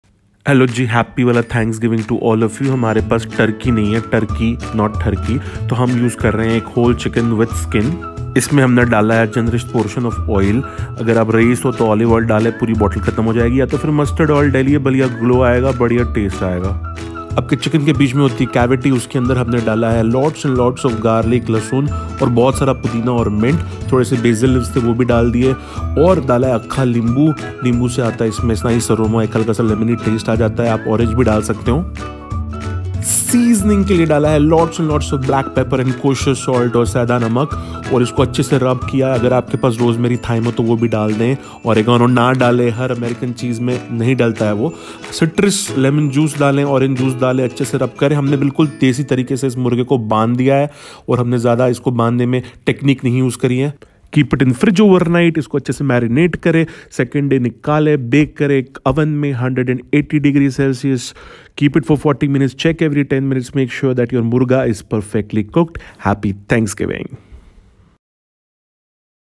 𝐓𝐡𝐞 𝐃𝐞𝐬𝐢 𝐓𝐡𝐚𝐧𝐤𝐬𝐠𝐢𝐯𝐢𝐧𝐠-⁣ Whole Chicken Roast
𝐓𝐡𝐞 𝐃𝐞𝐬𝐢 𝐓𝐡𝐚𝐧𝐤𝐬𝐠𝐢𝐯𝐢𝐧𝐠-⁣
Whole Chicken Roast  - east desi Jugad by Hippiefoodster. ⁣
- Buy Whole chicken ( Broiler) with skin bigger the bird more fun ⁣
- Dry it with a clean cloth ,remove all moisture!⁣
- Pour mustard oil and massage the chicken well covering each part inside out ⁣
- Sprinkle lots of kosher salt or black salt and fresh black pepper powder on the chicken and massage again ⁣
- Add lemon or orange juice now and massage  more ⁣
- Add rosemary and thyme if you have it !⁣
- Cavity you can fill it with lots of garlic, mint and basil leaves and whole lemon or orange. ⁣
- Tie the bird firmly with a thread, lot of complicated tricks on you tube.. but it doesn’t matter as you have to cut and throw that thread out eventually, you just need to tuck the chicken tightly so it won’t move and cook evenly ⁣
- Sprinkle mint leaves, basil leaves and garlic all over the bird  ⁣
- Overnight marinate the bird in fridge ⁣
- Take a baking tray and add veggies of your choice potatoes, onions, carrot and garlic will go well  - sprinkle some oil and salt and pepper and transfer the marinated bird on top. Make sure bird is at room temperature!⁣
- Preheat oven to 180 degrees and cook for 10- 15 mins. Turn the bird and cook for another 10 mins  - 15 mins. Just insert knife and keep checking your bird. It should not be overcooked! Keep it on grill mode on both side to make sure skin is crisp! ⁣
Cut the chicken with kitchen scissors keep the anatomy skills for Americans:) ! Desi wala thanksgiving murga ready. ⁣
𝗪𝐚𝐭𝐜𝐡 𝐯𝐢𝐝𝐞𝐨 𝐚𝐧𝐝 𝐦𝐚𝐤𝐞 𝐢𝐭 𝐭𝐡𝐢𝐬 𝐂𝐡𝐫𝐢𝐬𝐭𝐦𝐚𝐬 🎄 ❤️⁣